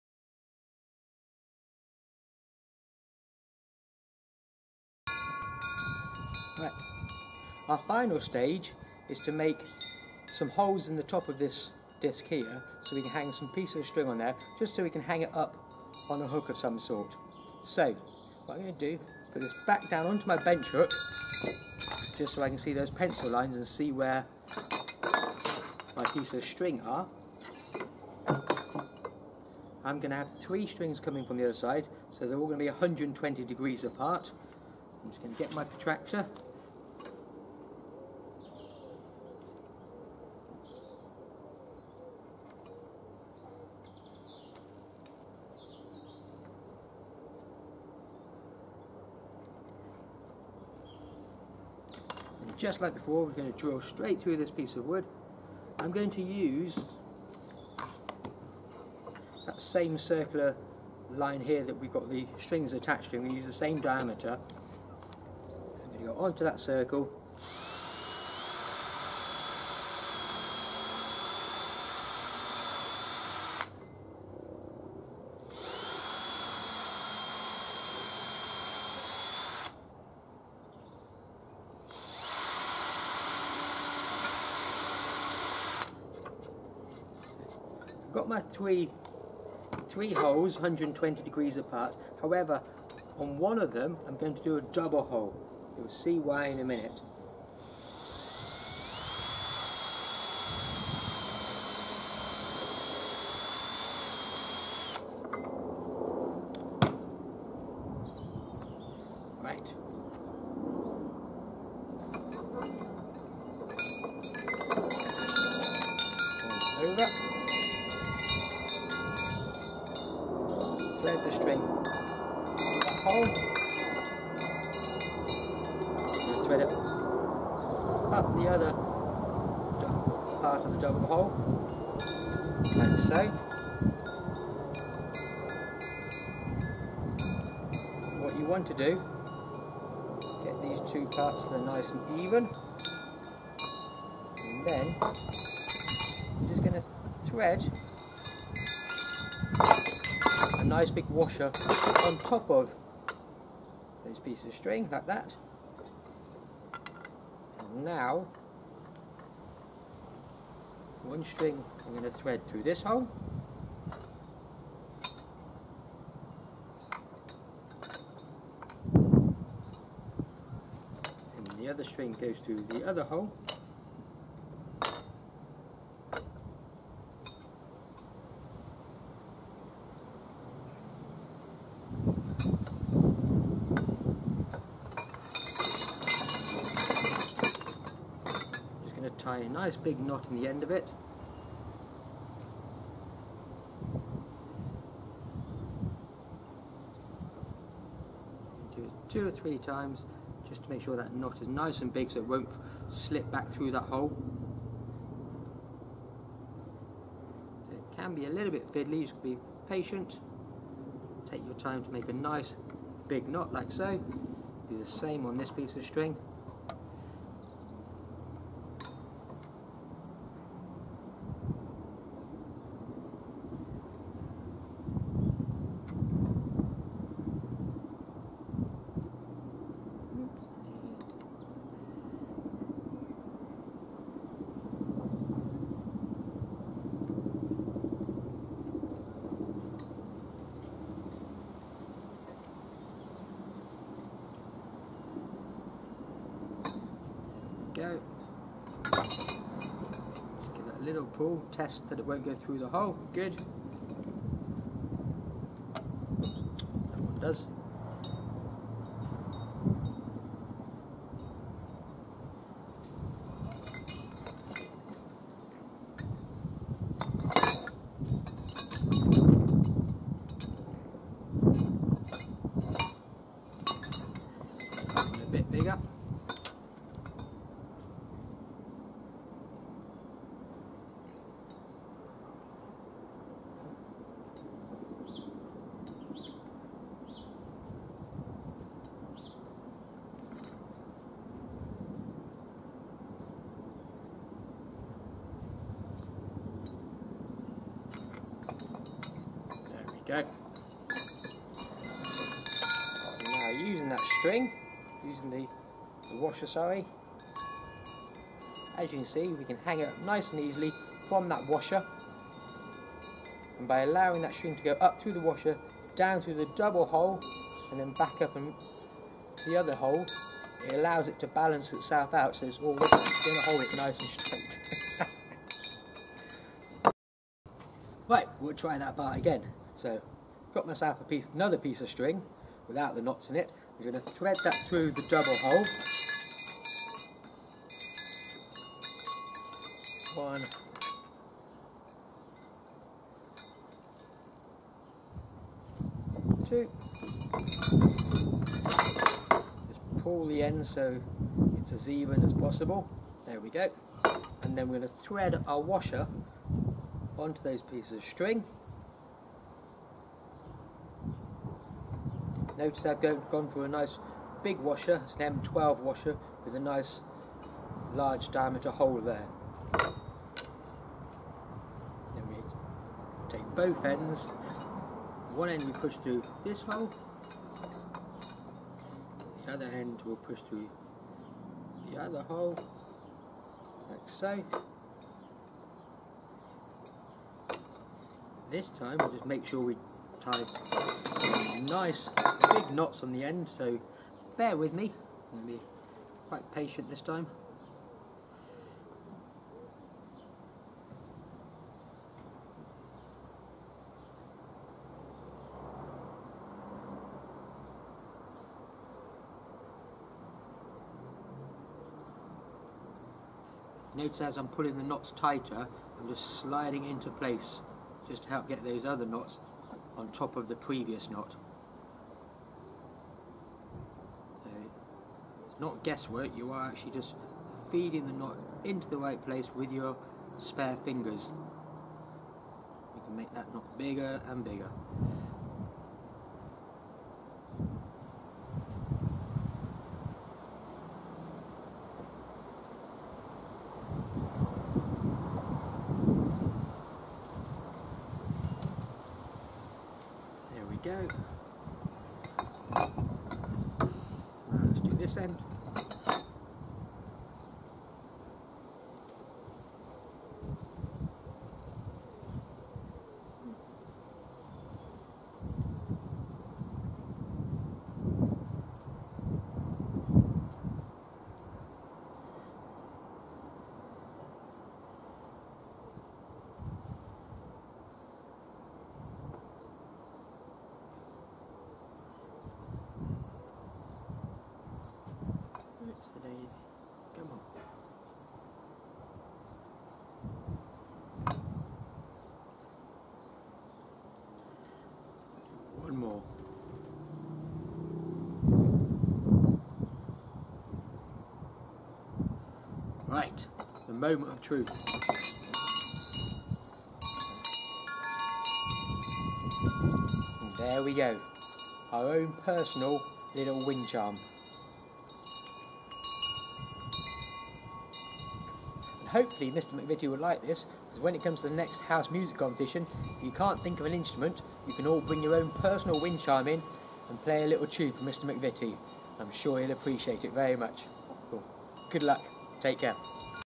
Wind Charm stage4
Woodworking for children
Designed for children between the ages of 7 years old and 13 years old.
Second stage in making your Wind Charm using copper piping.